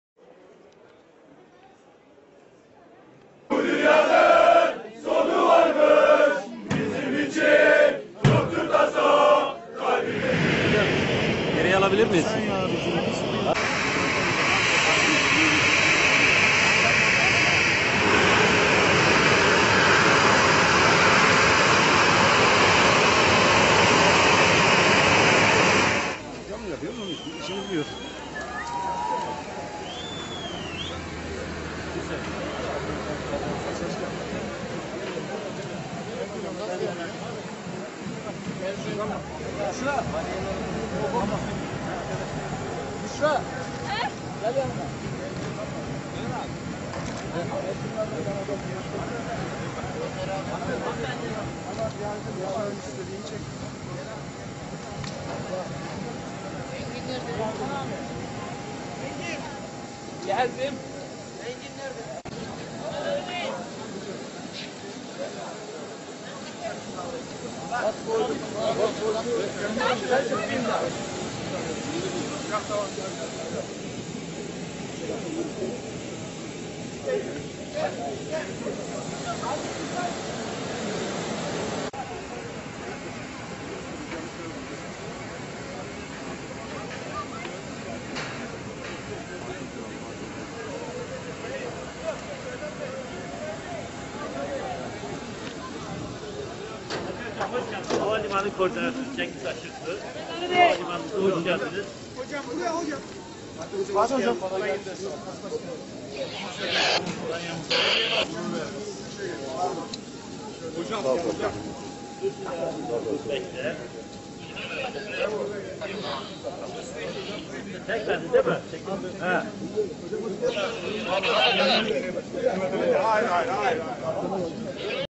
Galatasaray Gazipaşa'da coşkuyla karşılandı
Galatasaray Spor Kulübü kafilesi, devre arası kampına başlamak üzere bugün İstanbul'dan Antalya Gazipaşa Havalimanı'na uçtu. Pegasus Havayolları'na.